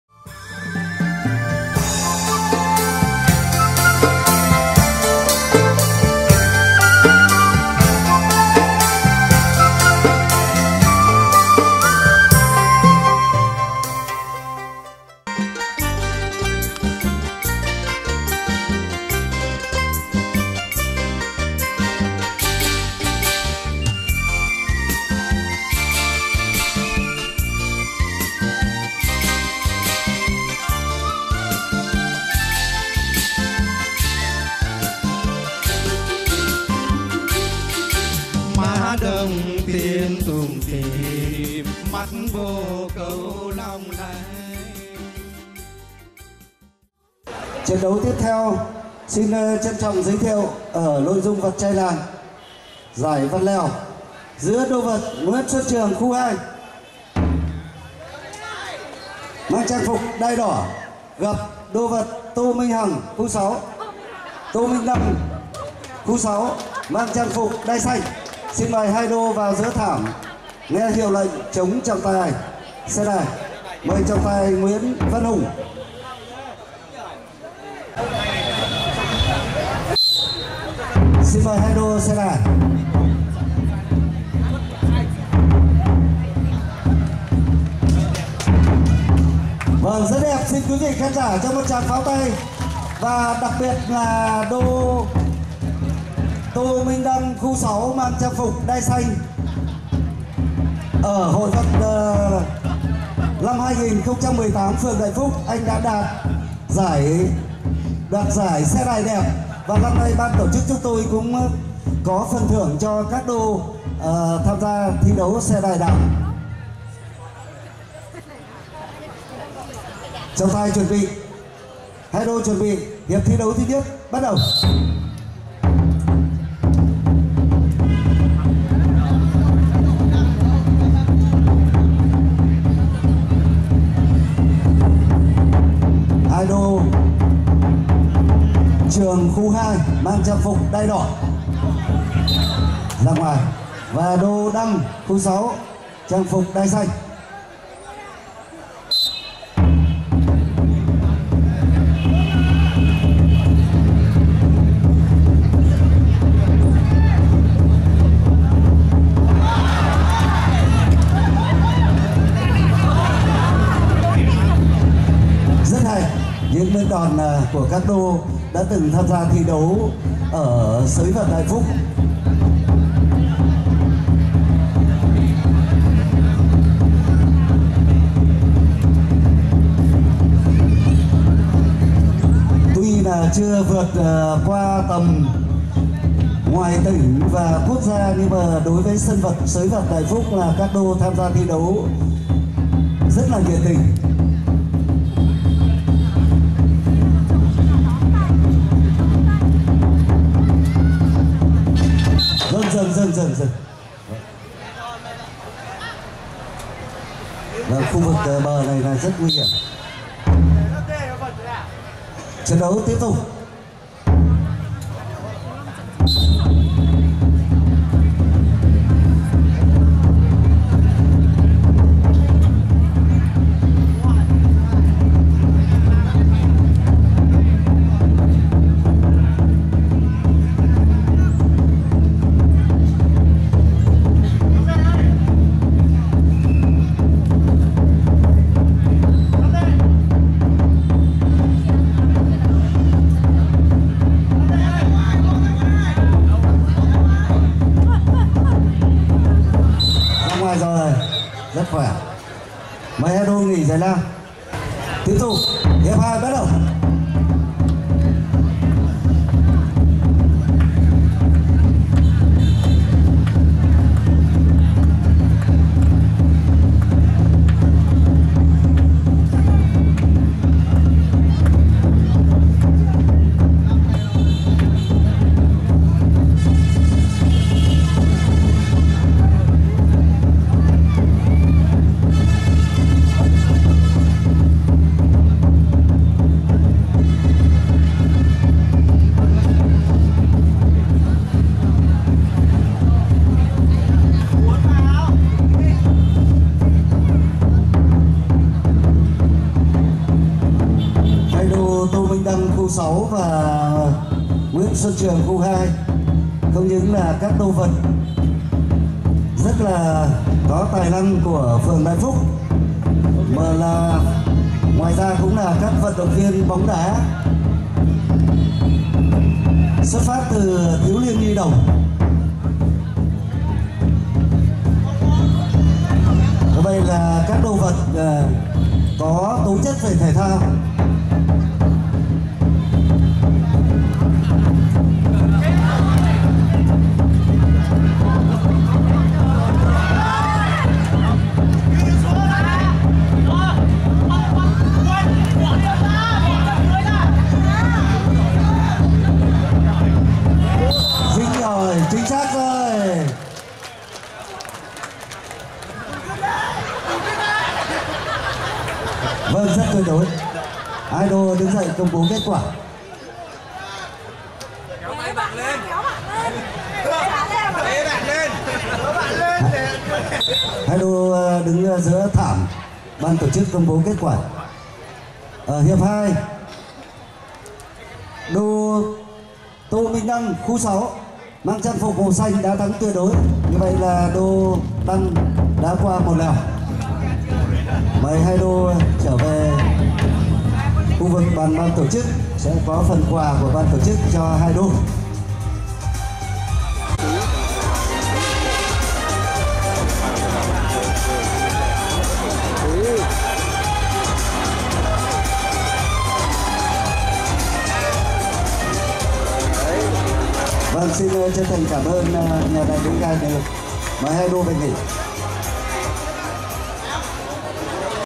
Đẳng cấp - Trai làng - Hội vật truyền thống Đại Phúc Bắc Ninh Xuân Kỷ Hợi 2019
- Hội vật truyền thống Đại Phúc Bắc Ninh có từ xa xưa, được tổ chức hàng năm vào 3 ngày: 8,9,10 tháng Giêng hàng năm, hội vật Đại Phúc nổi tiếng khắp vùng Kinh Bắc từ rất lâu rồi, chính vì sự nổi tiếng ở mức độ quy mô nên đã quy tụ được rất nhiều các đô vật nổi tiếng từ khắp nơi về tham dự...
- Quý vị hãy ghé thăm hội vật truyền thống Đại Phúc Bắc Ninh Xuân Kỷ Hợi 2019 để được đắm chìm trong không khí tưng bừng, náo nhiệt của những keo vật kinh điển trên quê hương Quan họ nhé!